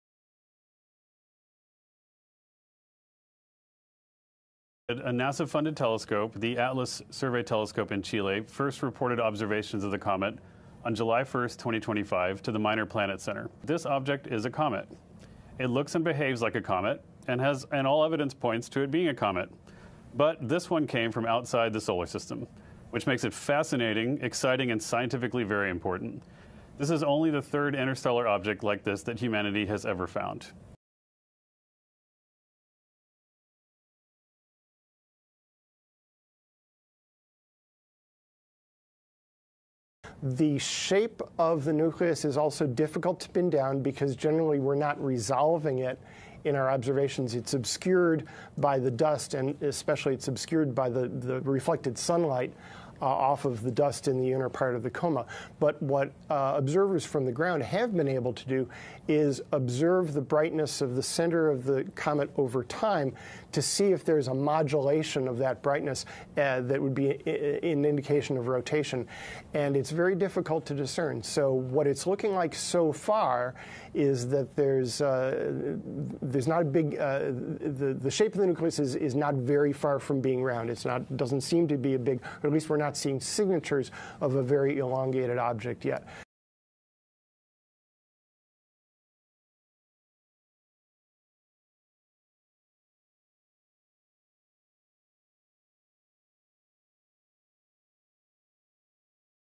La NASA pubblica nuove immagini della cometa interstellare 3I/ATLAS
La NASA ha diffuso immagini ravvicinate della cometa interstellare 3I/Atlas, il terzo oggetto noto proveniente da un’altra stella. Dopo il passaggio vicino a Marte, a metà dicembre si avvicinerà alla Terra prima di tornare nello spazio interstellare.